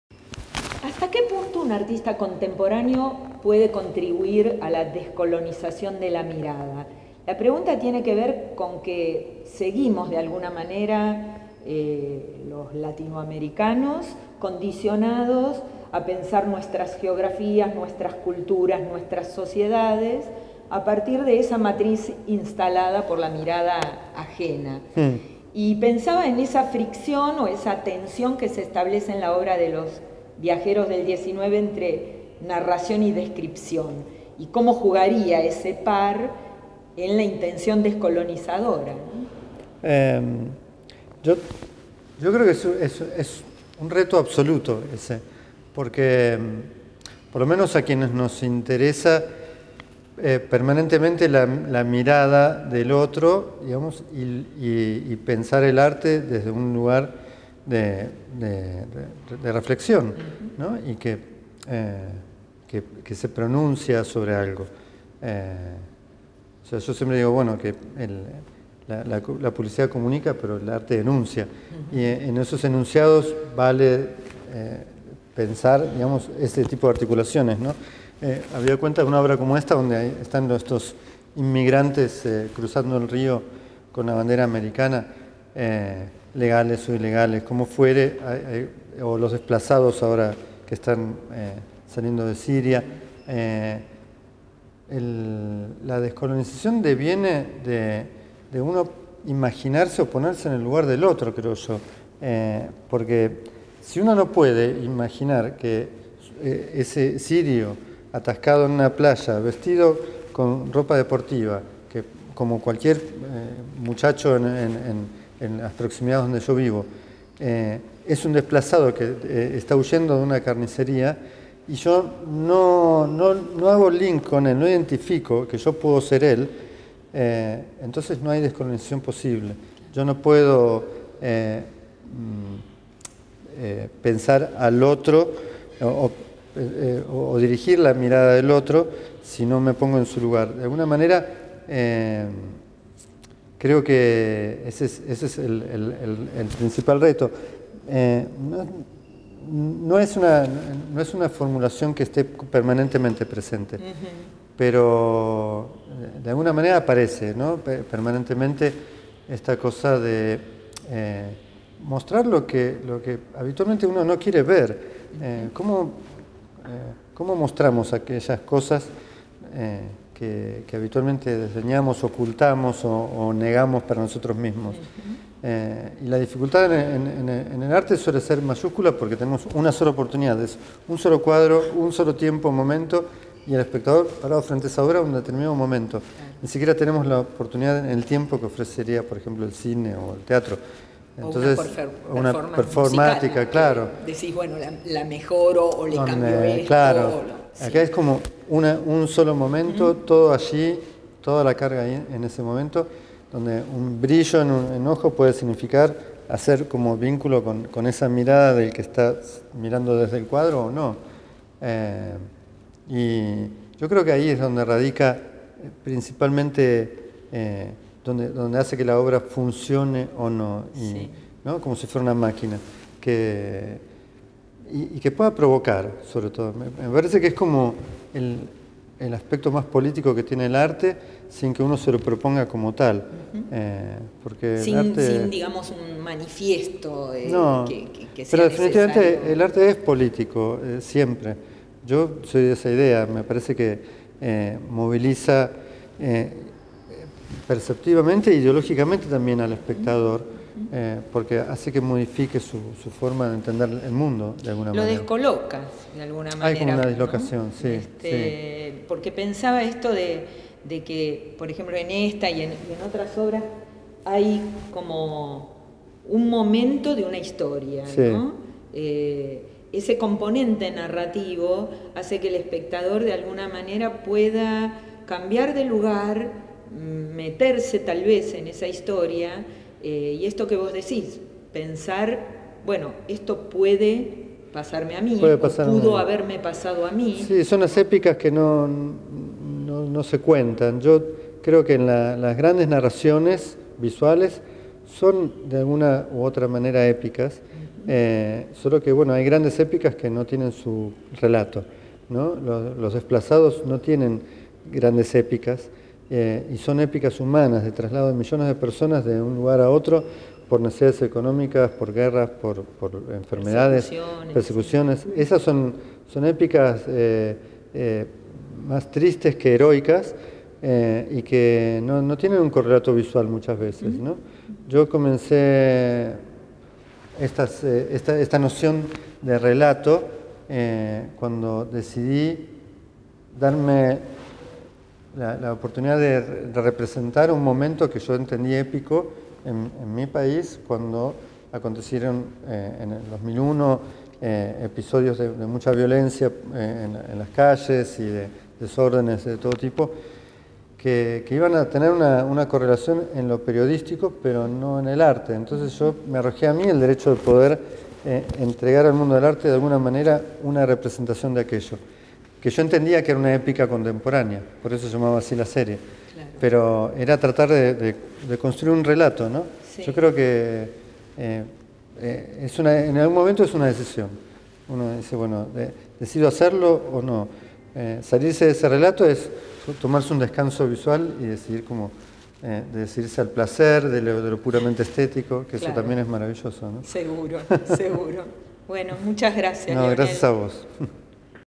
Entretiens d'Artelogie à Leonel Luna (5)
Entrevista del pintor Leonel Luna por la historiadora del arte Marta Penhos a propósito de la exposición Paisaje Americano. Curaduría: Cecilia Cavanagh. Pabellón de las Artes, Universidad Católica Argentina, Buenos Aires, septiembre – octubre 2016. Video: Pablo Gaiazzi, Media lab.